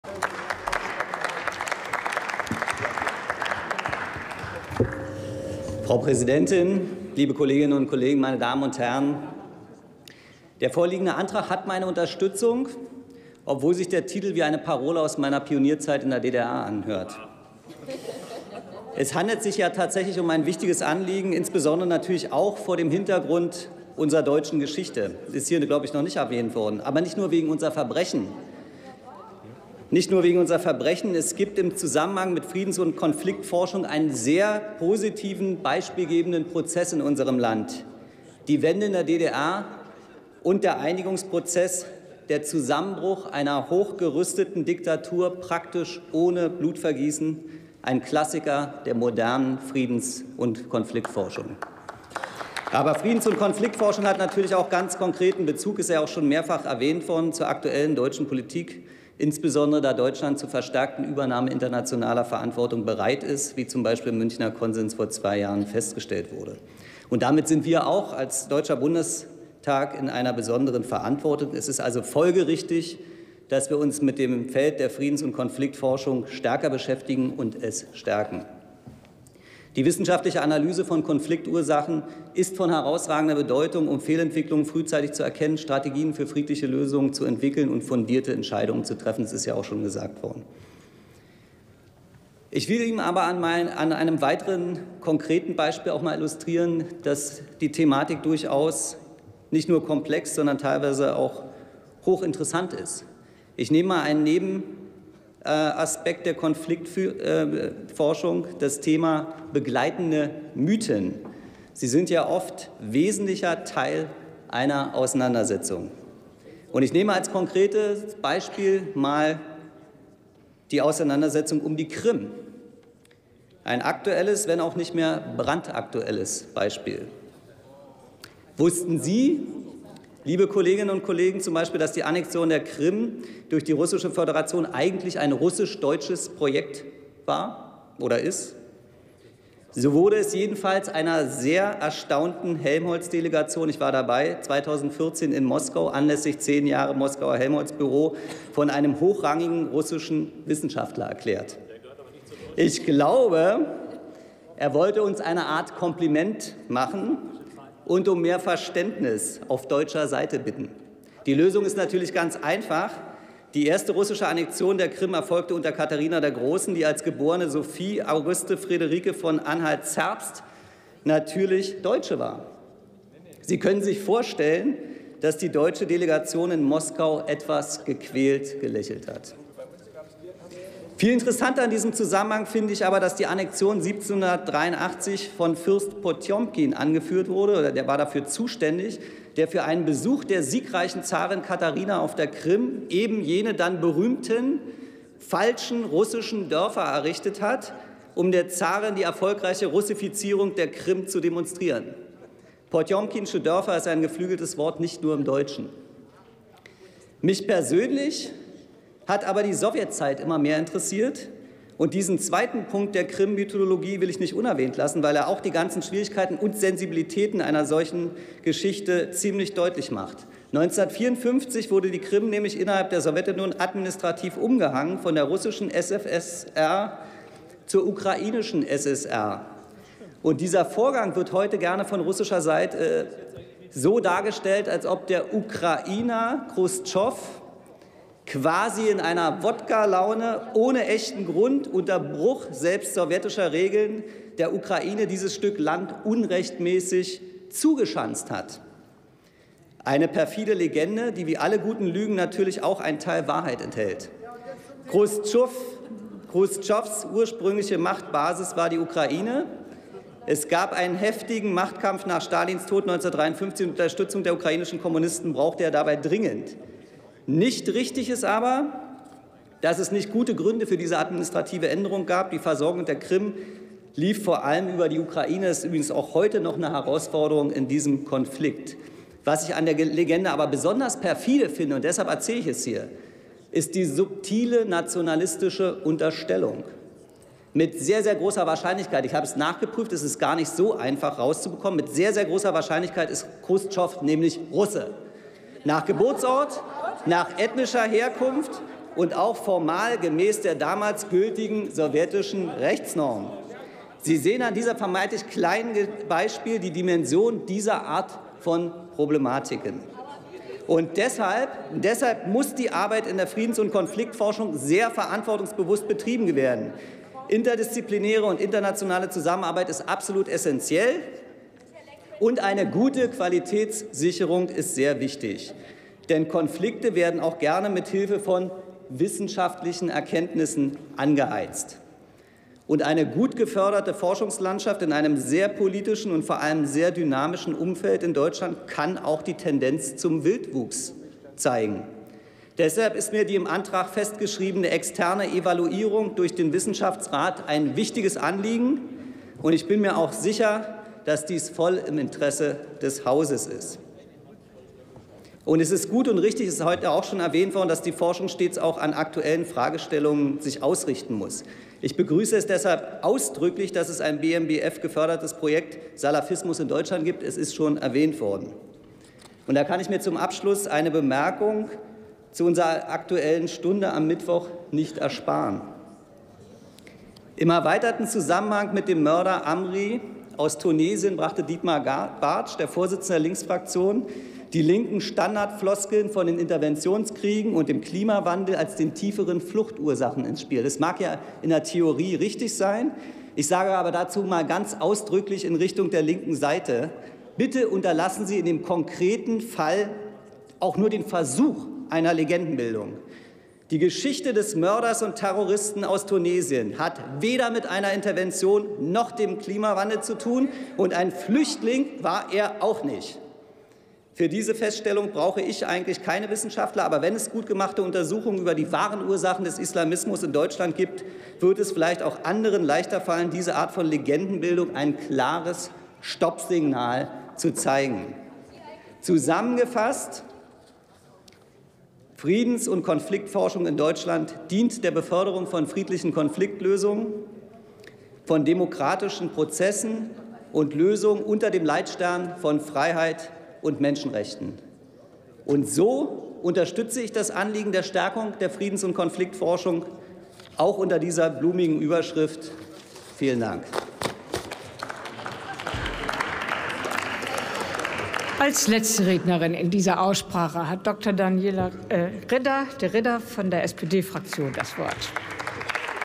Philipp Lengsfeld: Friedens- und Konfliktforschung [Bundestag 20.01.2017]
Beratung der Beschlussempfehlung und des Berichts des Ausschusses für Bildung, Forschung und Technikfolgenabschätzung (18. Ausschuss)
zu dem Antrag der Fraktionen der CDU/CSU und SPD
Dem Frieden verpflichtet – Friedens- und Konfliktforschung stärken

18. Deutscher Bundestag
213. Plenarsitzung
Tagesordnungspunkt: Friedens- und Konfliktforschung
Redner: Lengsfeld, Dr. Philipp, CDU/CSU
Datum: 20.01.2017
Plenarprotokoll:
UVID:7062028